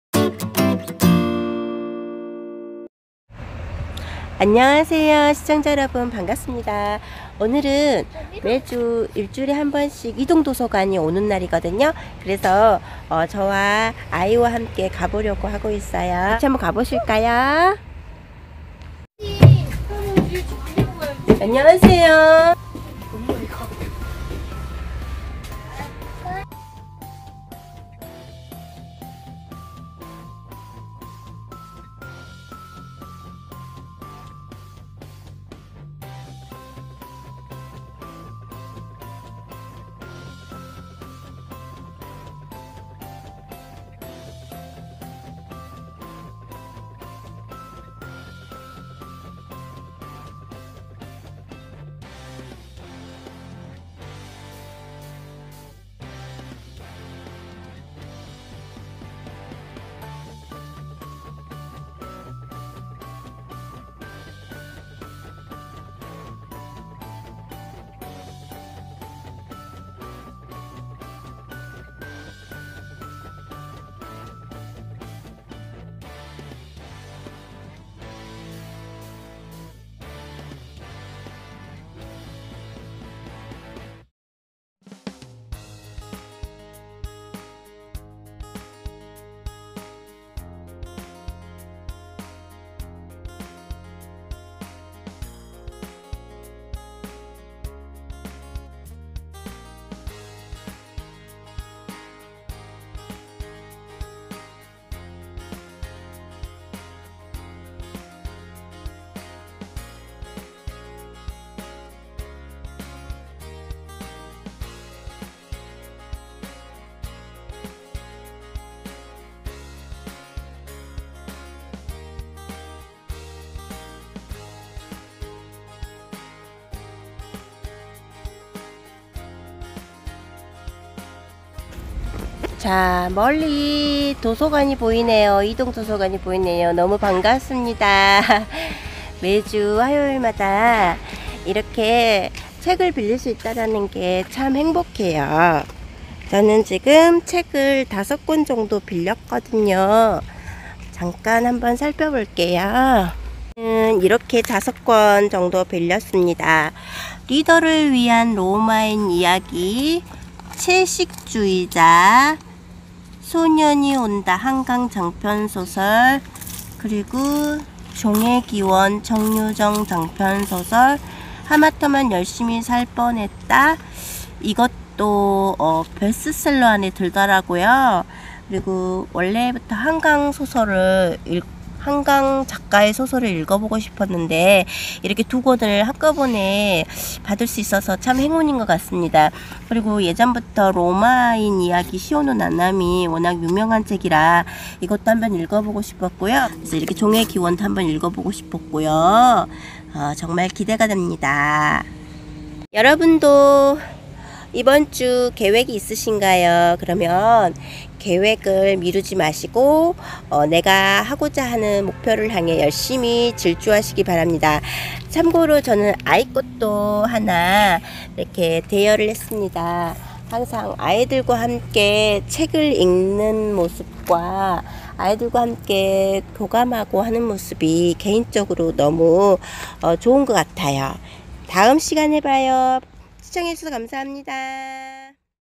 [자기계발#8]이동도서관 책도보고 좋네요 독서즐거움Thanks to the mobile library, it's nice to read books! reading pleasure
안녕하세요
두아이의 엄마이자 간호사 공코입니다
매주 오는 이동도서관 차 덕분에 책읽는 즐거움을 더욱 업그레이드 해요
함께 가보아요

Hello
I'm Gongko, the mother and nurse of two children.
Thanks to the weekly mobile library car, I can upgrade the pleasure of reading.
Let's go together.
你好。
我是两个孩子的妈妈兼护士 孔科
多亏每周来的流动图书馆的车，读书的乐趣更上一层楼了
一起走吧
二人の子供のお母さんで看護師のコンコです。
毎週訪れる移動図書館の車のおかげで本を読む楽しさをさらに向上させます。
いっしょに行きましょう